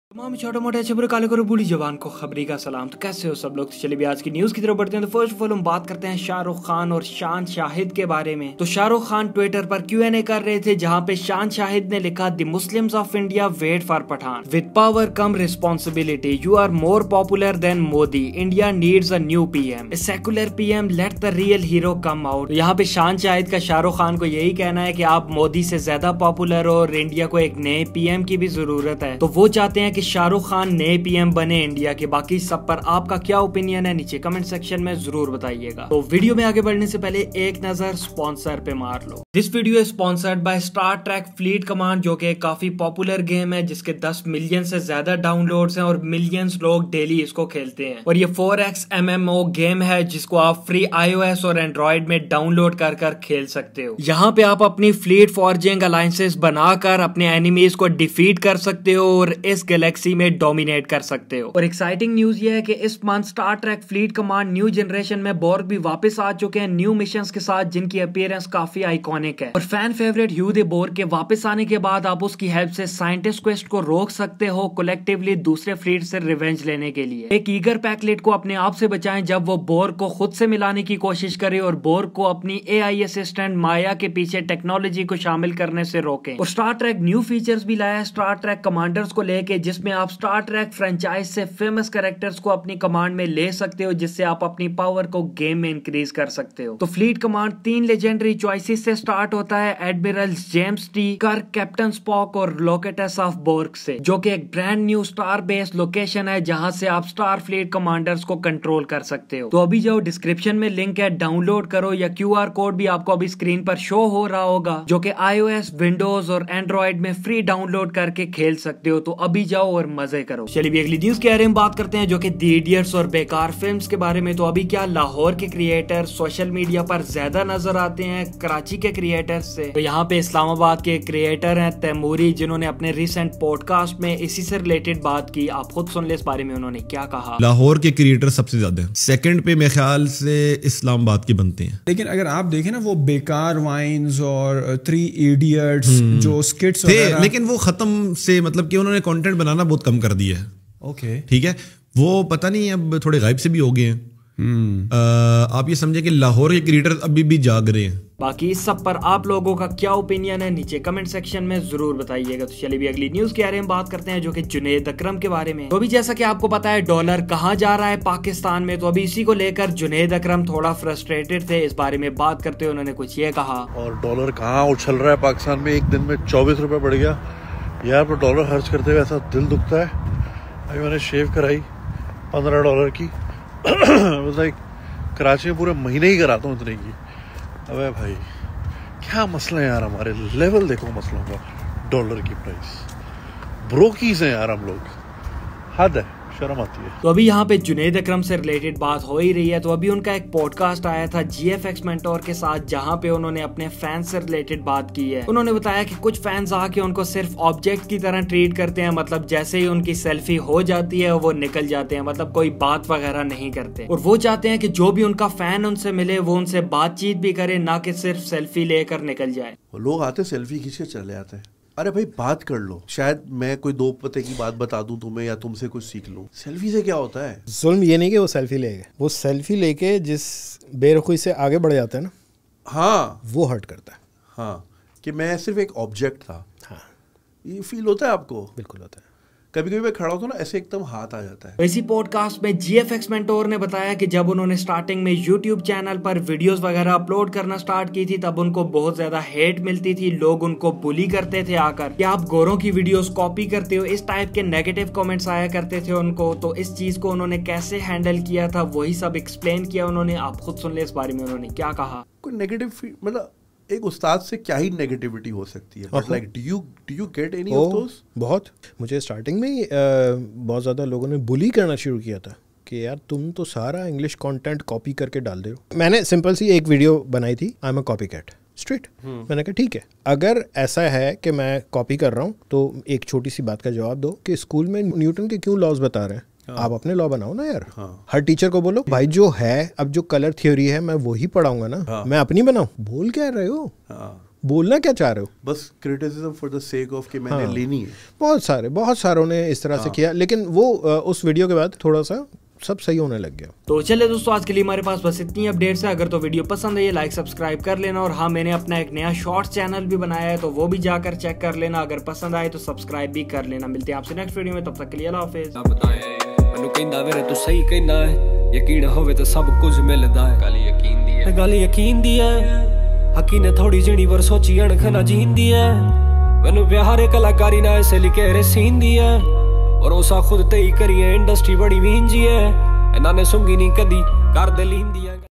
Shaan Shahid Reply Shah Rukh Khan | Gfx Mentor Bullied? | Junaid Akram Hurt By Fans
Khabri YT